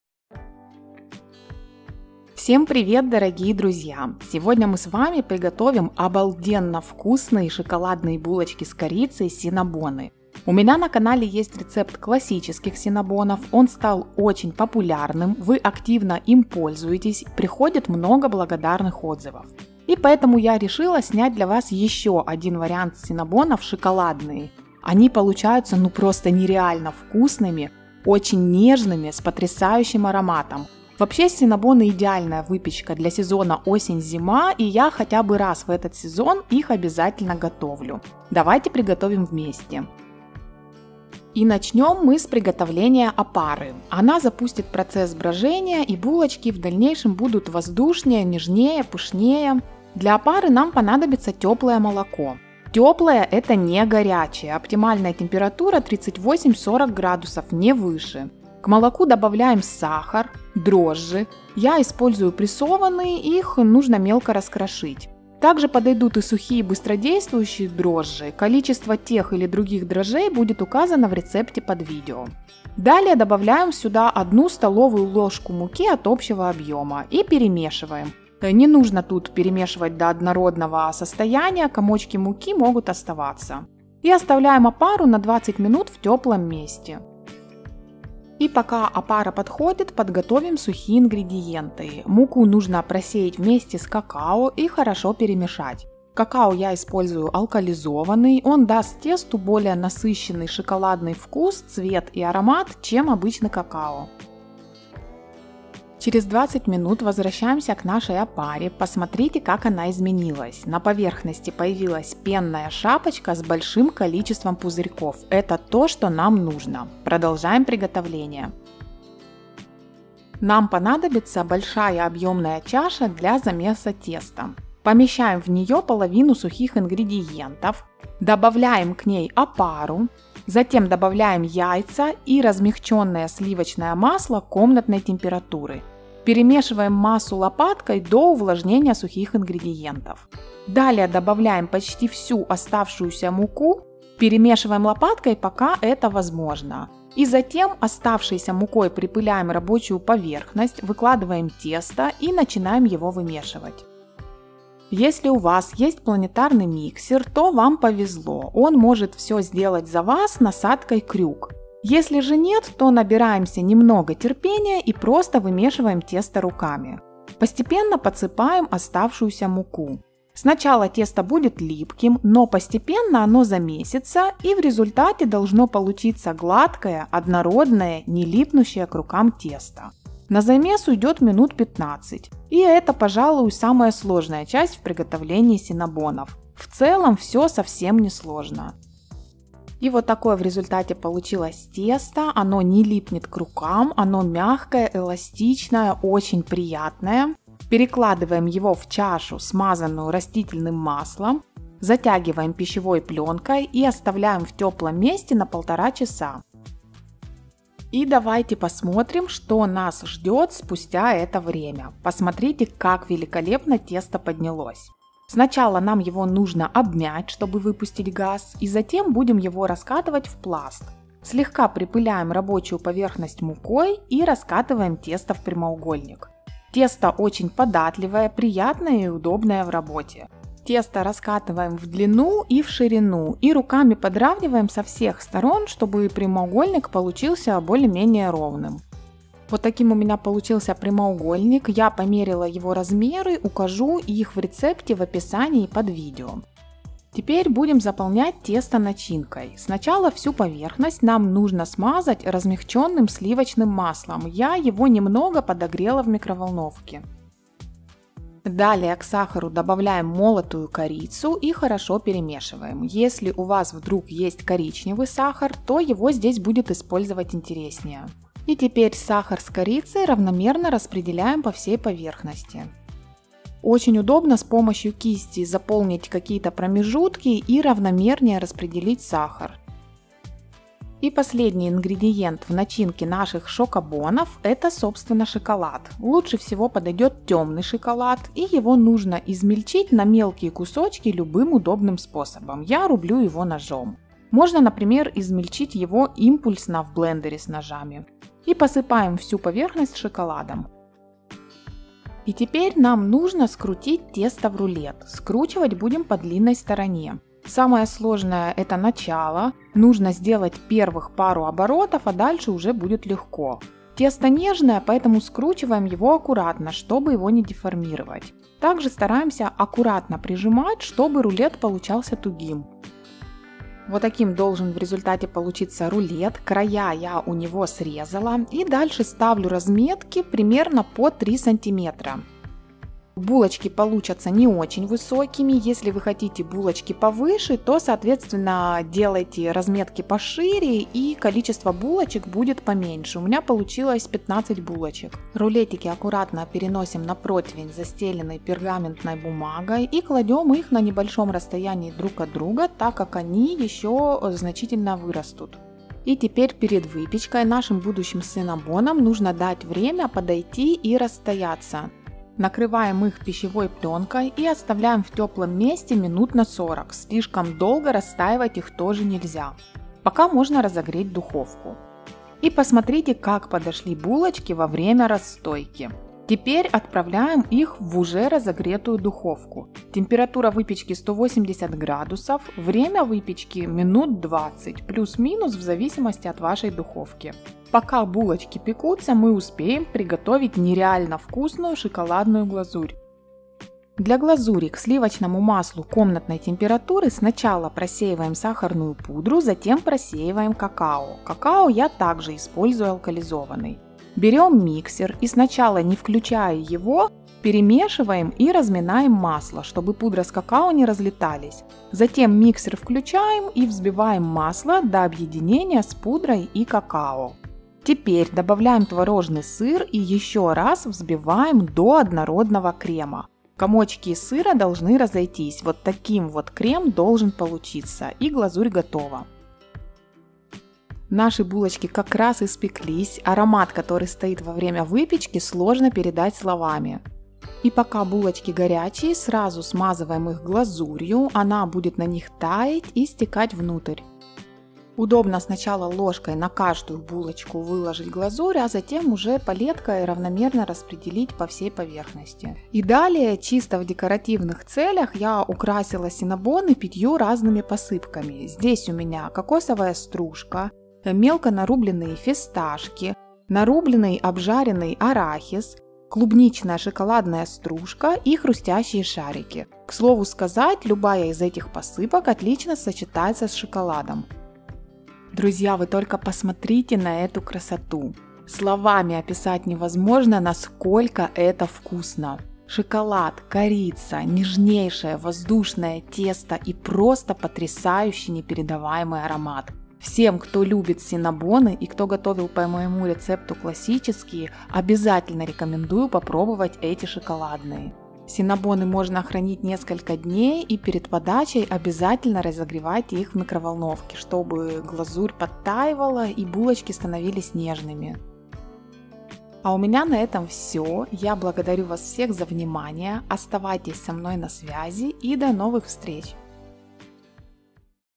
Шоколадные СИННАБОНЫ рецепт / БУЛОЧКИ с корицей
РЕЦЕПТ невероятно вкусных ШОКОЛАДНЫХ СИННАБОНОВ! На моем канале есть РЕЦЕПТ классических СИННАБОНОВ, он стал популярным, вы часто по нему готовите. А теперь обязательно попробуйте шоколадные СИННАБОНЫ! Это бомба!!!
Булочки можно брать с собой в гости или делать на заказ. А готовятся они совсем  несложно!
ЗАПРЕЩЕНО КОПИРОВАНИЕ И ИСПОЛЬЗОВАНИЕ ВИДЕО-МАТЕРИАЛОВ, в том числе РАЗМЕЩЕНИЕ НА СТОРОННИХ РЕСУРСАХ БЕЗ СОГЛАСИЯ АВТОРА КАНАЛА

Тесто

Молоко 250 г (38-40 С)
Дрожжи 24 г прессованные или 8 г сухие быстродействующие
Какао алк.50 г
Яйца 100 г (2 шт С1)

Начинка

Сливочное масло 60 г (размягченное)
Сахар 120 г
Корица 25 г (3 ст.л.)
Темный шоколад 100 г (нарубить крошкой)

Глазурь

Какао алк.20 г
Творожный сыр 100 г

Размер пласта теста 30*60 см
Противень 35*25 см